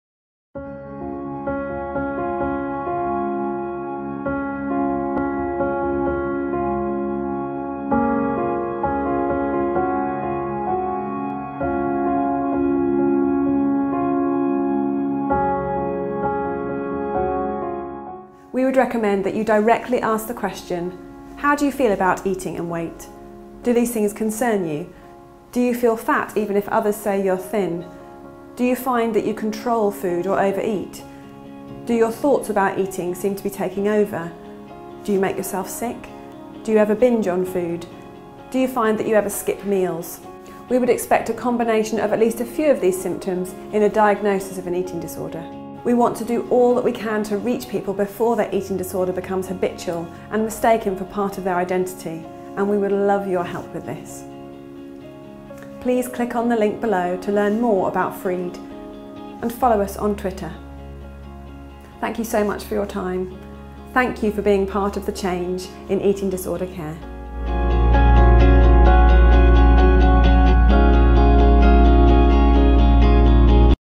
Eating Disorder Information for GPs from FREED (Social Media Clip 2) LYPFT
A resource developed for GPs to inform them about the early signs of an eating disorder, and to support them to reach out to the people they support who may be struggling with an eating disorders.